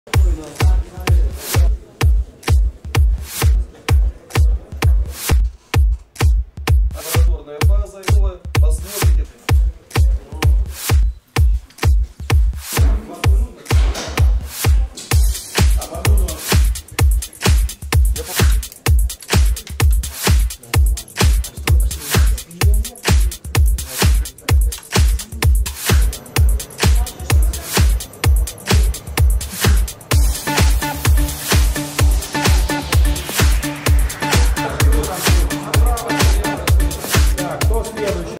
Тактические тренажеры в БНТУ
Современные тренажеры стрелкового оружия и новейший тактический симулятор появится в следующем учебном году на военно-техническом факультете Белорусского национального технического университета. Обучающий комплекс поможет курсантам совершенствовать тактику ведения боевых действий вплоть до батальонного звена прямо в учебной аудитории. Об этом и не только узнали будущие абитуриенты на дне открытых дверей в БНТУ.
Кстати, сегодня военно-технический факультет -- один из самых высокотехнологичных в Беларуси: все обучение будущих офицеров строится на работе с тренажерными средствами и системами виртуализации.